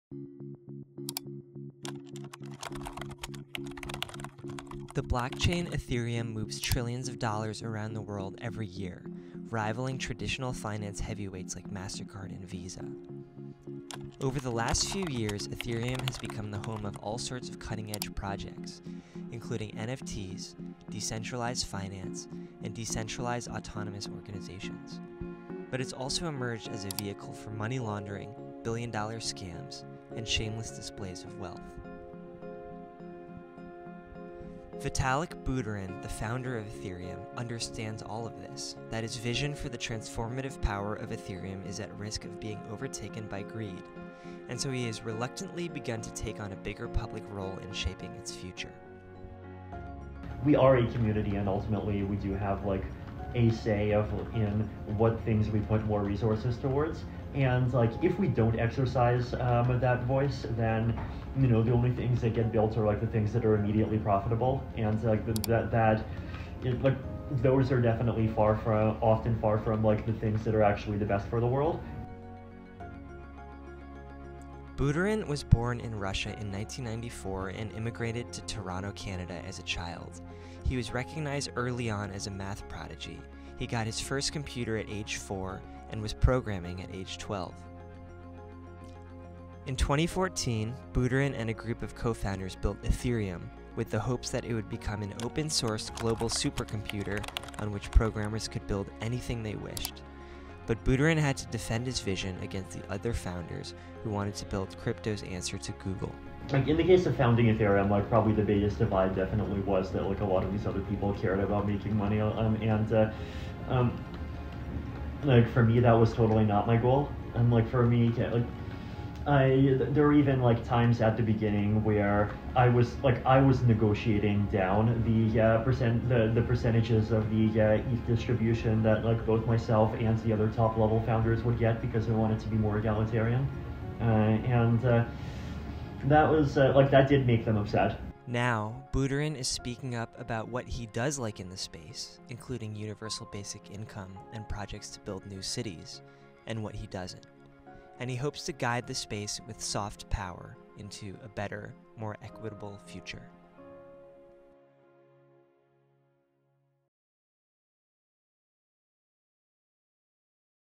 Vitalik Buterin on the Transformative Power of Ethereum
Vitalik Buterin, the most influential person in crypto, didn’t come to Denver to party. He doesn’t drink or particularly enjoy crowds. Not that there isn’t plenty for the 28-year-old creator of Ethereum to celebrate. Nine years ago, Buterin dreamed up Ethereum as a way to leverage the blockchain technology underlying Bitcoin for all sorts of uses beyond currency. Since then, it has emerged as the bedrock layer of what advocates say will be a new, open-source, decentralized internet. Ether, the platform’s native currency, has become the second biggest cryptocurrency behind Bitcoin, powering a trillion-dollar ecosystem that rivals Visa in terms of the money it moves. Ethereum has brought thousands of unbanked people around the world into financial systems, allowed capital to flow unencumbered across borders, and provided the infrastructure for entrepreneurs to build all sorts of new products, from payment systems to prediction markets, digital swap meets to medical-research hubs.